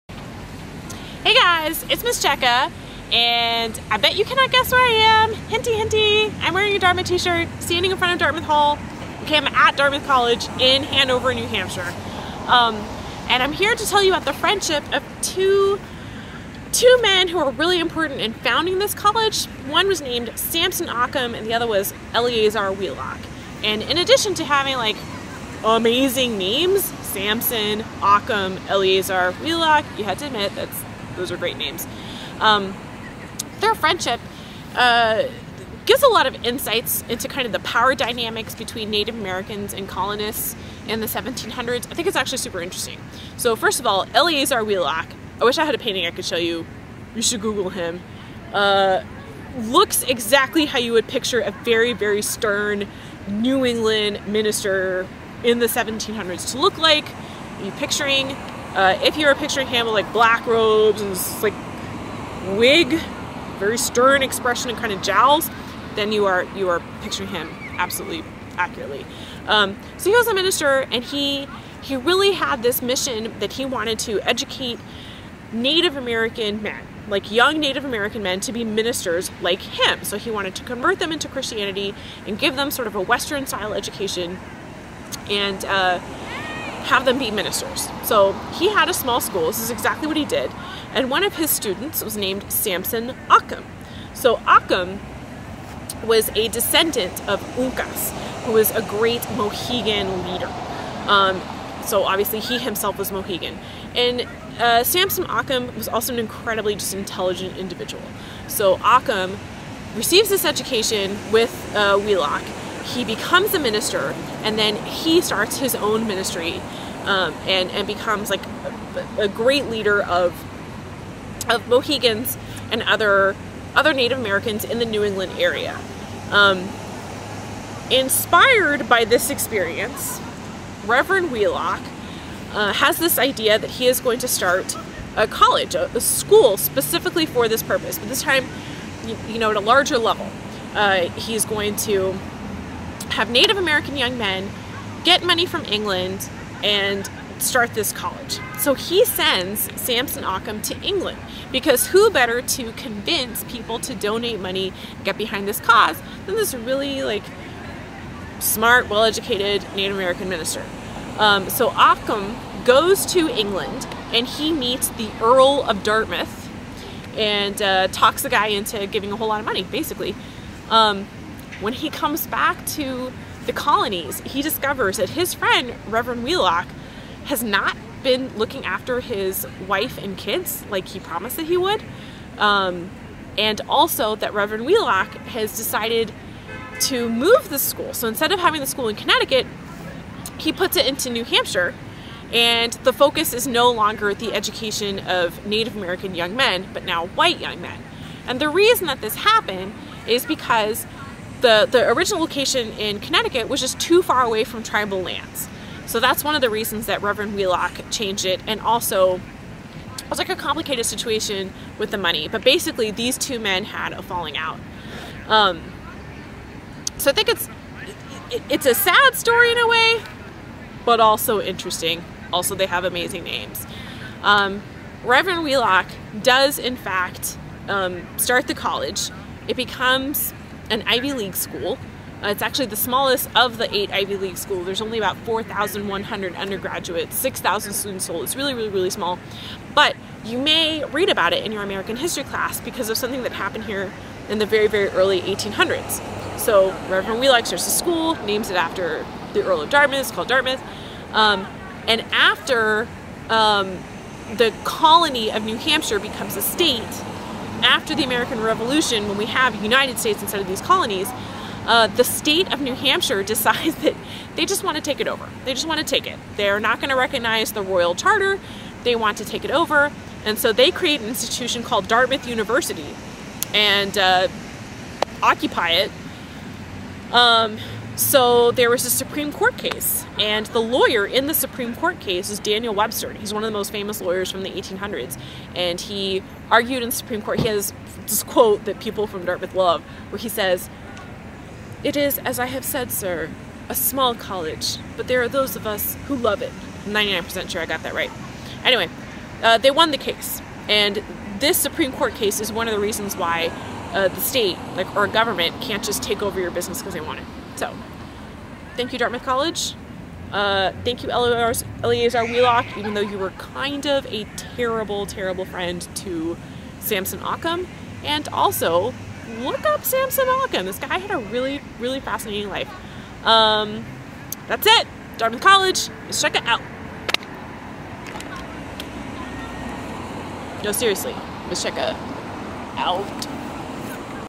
Ms. Checka Goes to Dartmouth
Ms. Checka tells the story of two frenemies, Samson Occum and Eleazar Wheelock